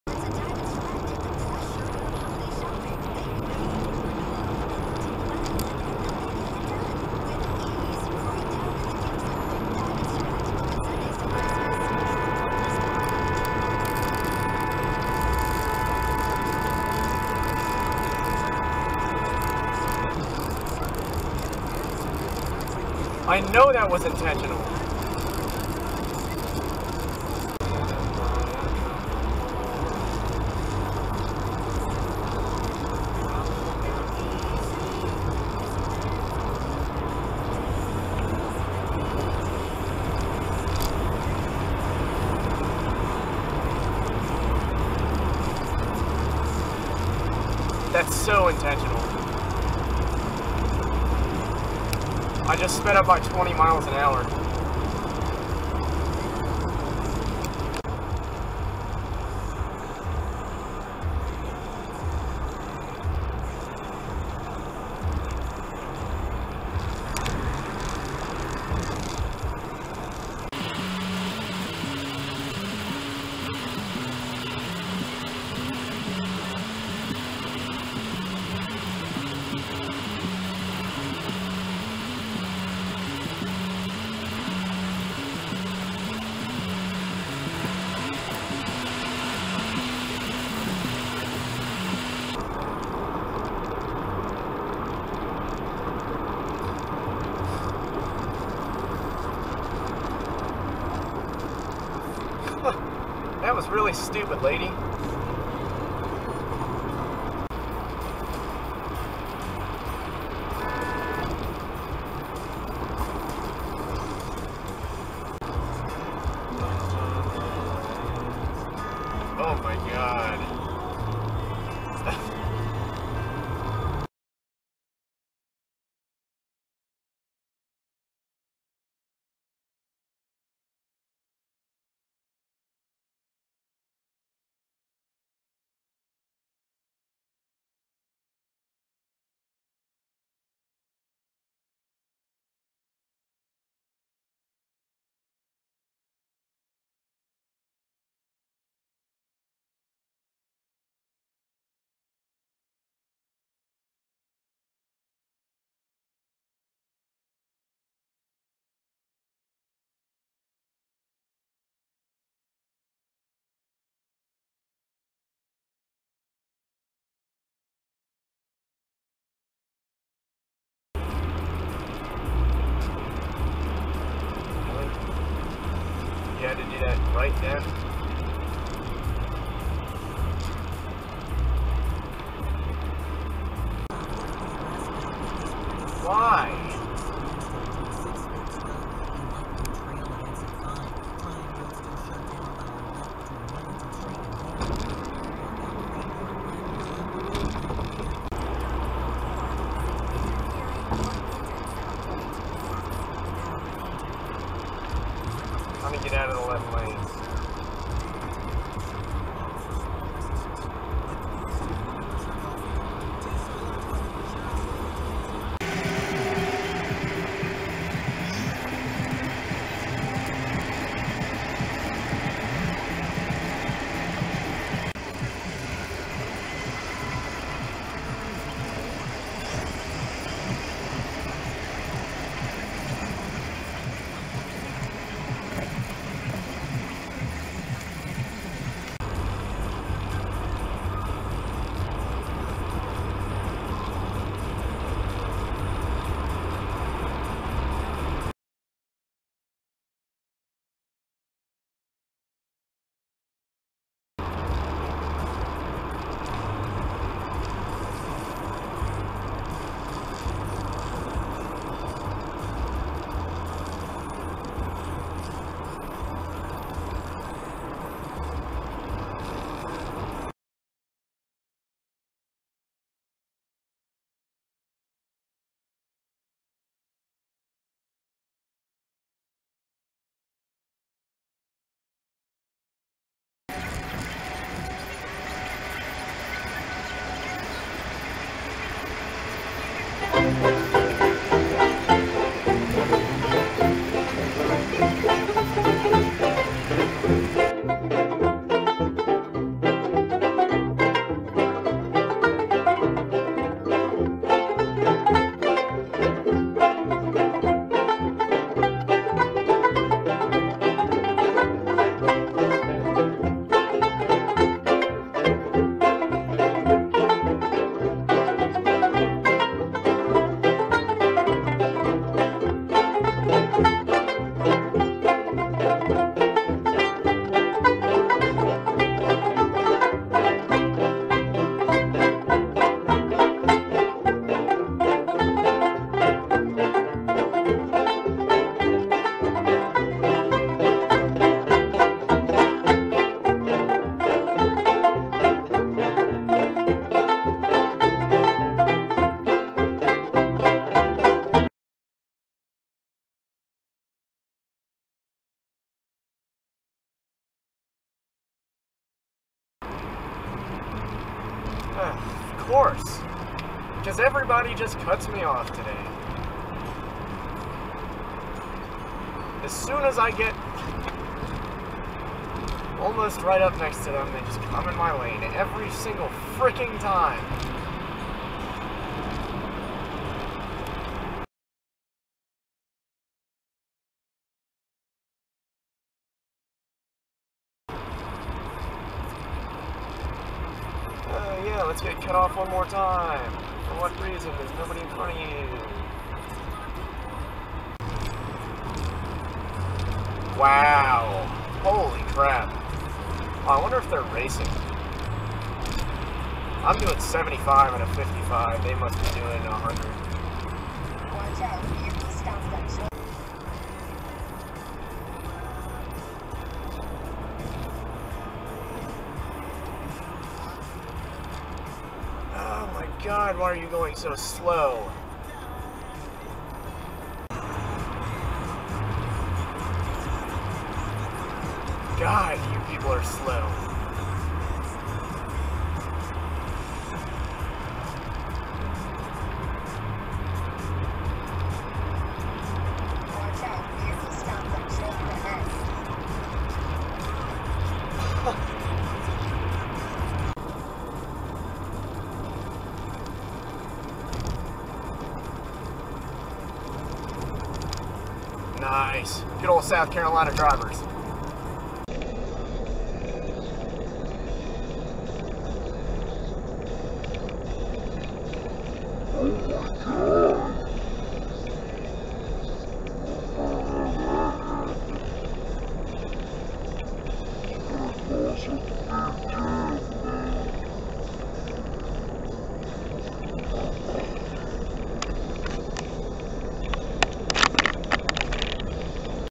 Clueless Drivers of Interstate 95 #3
Slowpokes, cutoffs and the road raging Kia Soul. I'll have a nice surprise for yall next weekend!

I'm not a perfect driver nor do I claim to be.

This is strictly for entertainment.

The camera tends to distort the real-life perception, be regardful of this when judging distance and speed.

Make sure you look up traffic laws for MD, VA, NC & SC before commenting or kindly ask before making accusations; it might be different from where you live.

Many of these aren't necessarily "bad drivers", just people who make honest mistakes which I have made in the past as well.